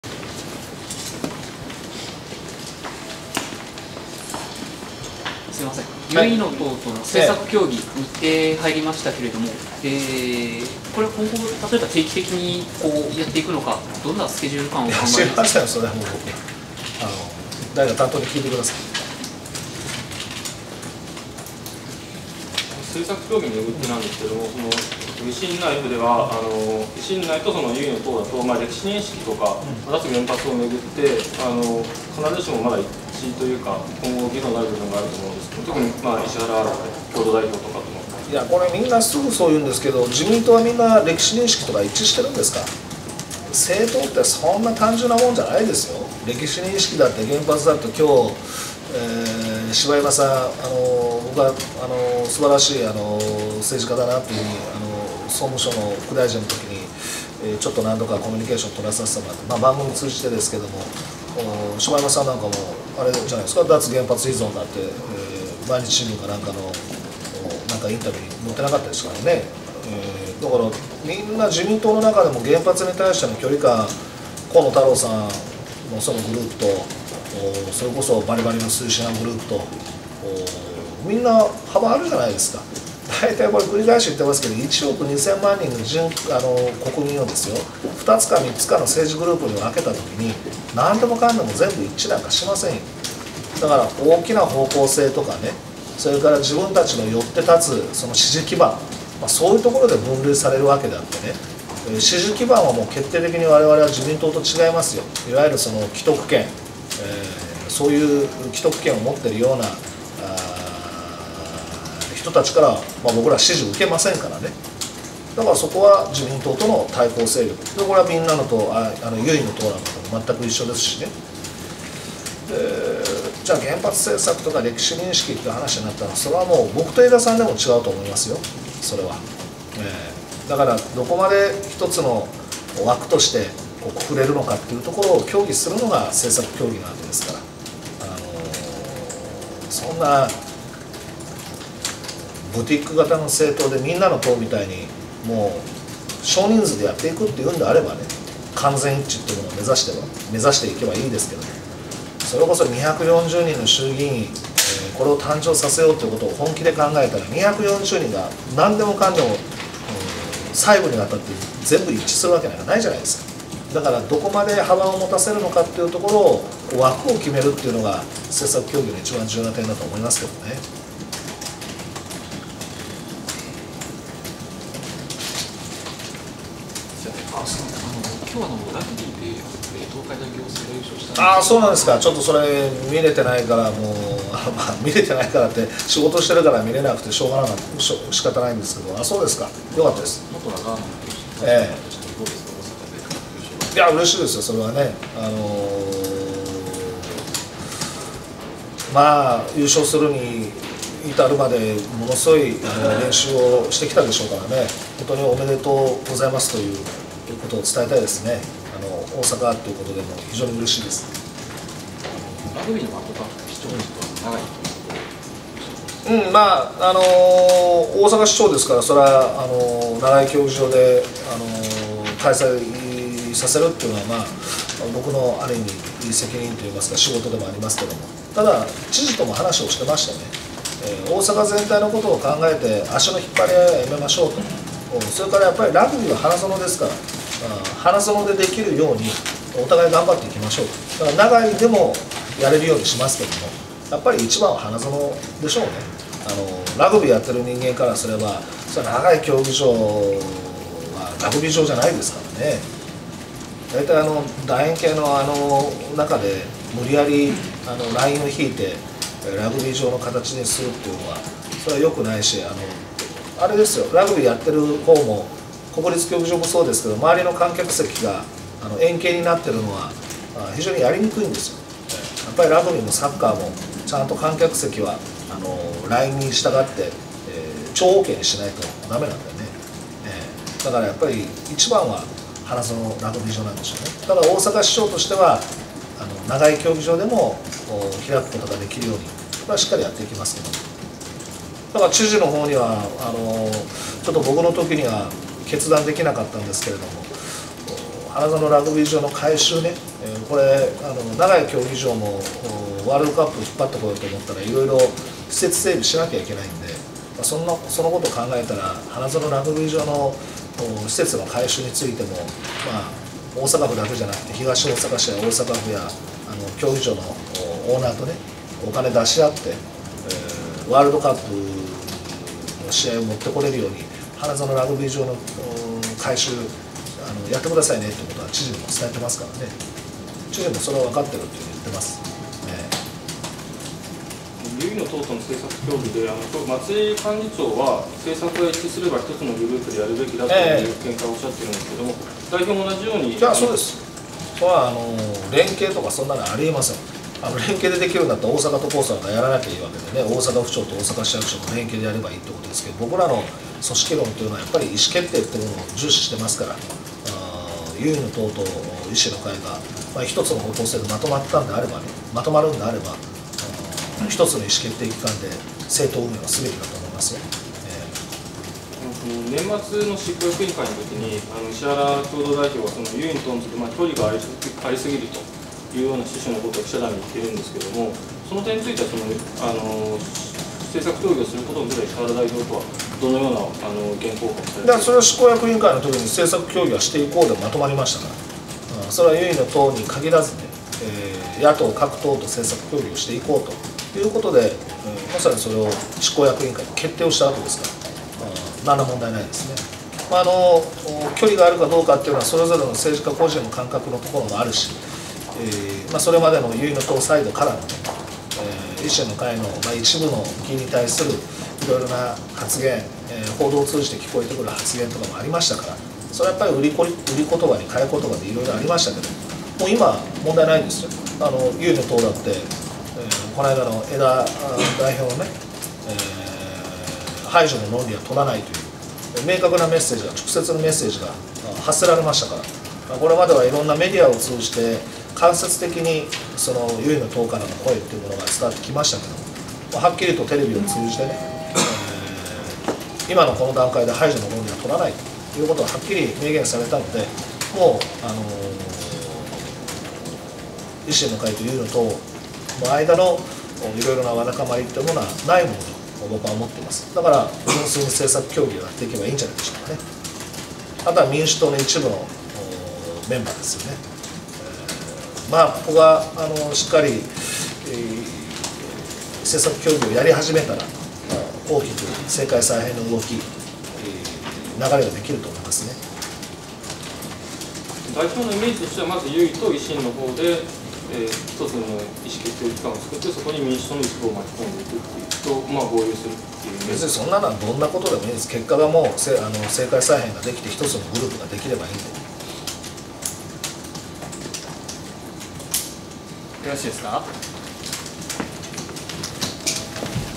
【2014.01.07】橋下徹 大阪市長 退庁時 ぶらさがり取材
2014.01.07（火）市役所退庁時の囲み取材の様子です。